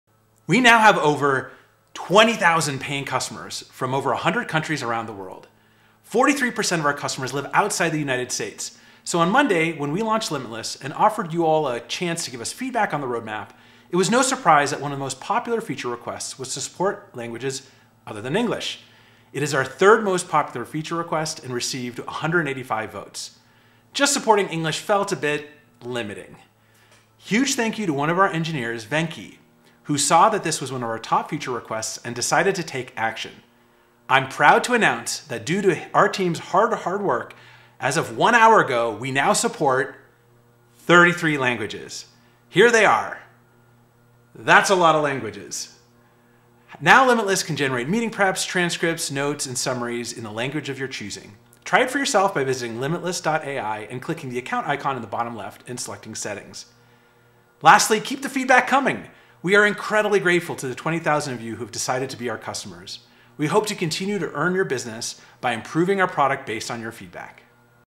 Limitless now supports 33 languages!
On Monday we launched Limitless: a personalized AI powered by what you've seen, said, or heard.

Since then a top feature request has been supporting more languages.

I'm proud of our team for being so customer centric and without any prompting from me they shipped support for 33 languages in just a few days!